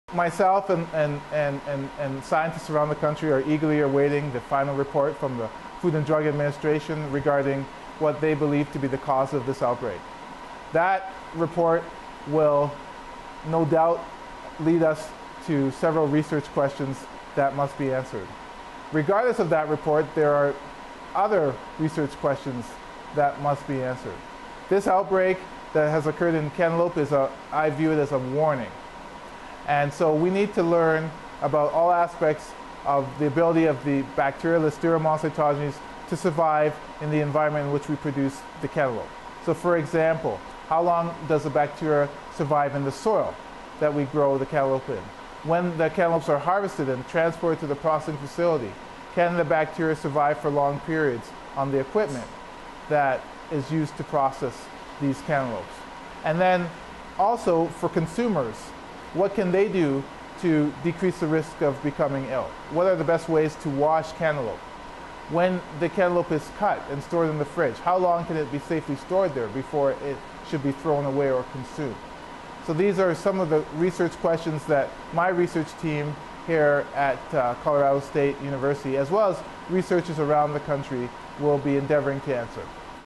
CSU food safety researchers investigate Listeria bacterium from farm to fork
As a deadly listeriosis outbreak causes nationwide concern, food safety experts at Colorado State University are launching a research project to better understand -- and prevent -- the spread of Listeria monocytogenes bacterium in cantaloupe. The research team will investigate the route of Listeria from farm to fork to help protect the cantaloupe industry and consumers from future outbreaks of contamination and illness. Lawrence Goodridge, a food microbiologist and associate professor in CSU's College of Agricultural Sciences, explains the project.